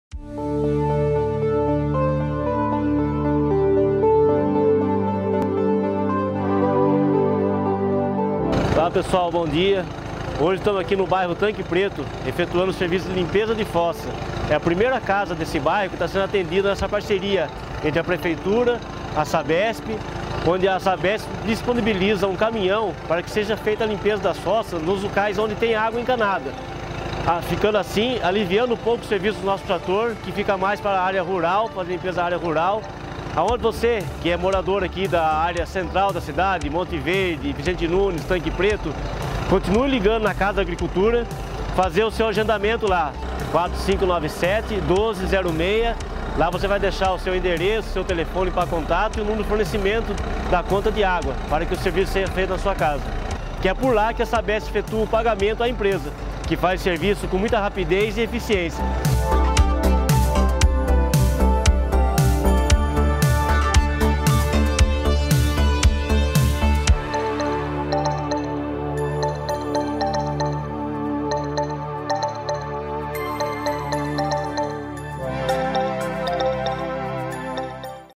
PREFEITURA REALIZA LIMPEZA DE FOSSA EM PARCERIA COM A SABESP
A prefeitura de Nazaré Paulista, através do departamento de Agricultura, executa parceria com a Sabesp para a limpeza de fossa nos locais onde tem água encanada.